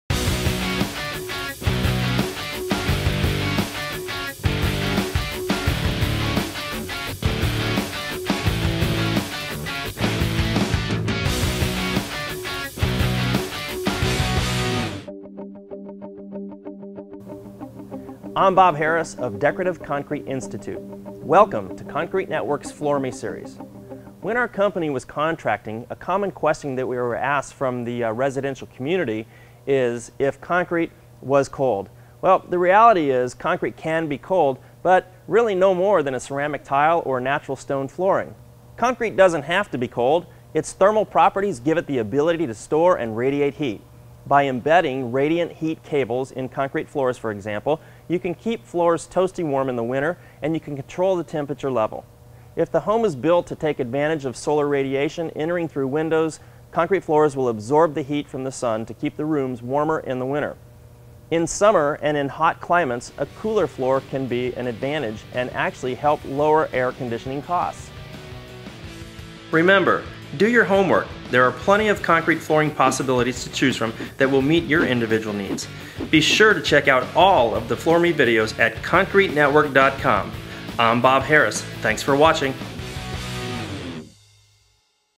Are Concrete Floors Cold?
Find out if concrete floors are cold and how people use concrete's properties to heat and cool their home. Homeowners and building owners often use radiant floor heating and cooling systems to control indoor temperatures.

Stay up-to-date: Sign up for the monthly Bob and Lee Ann Harris Newsletter: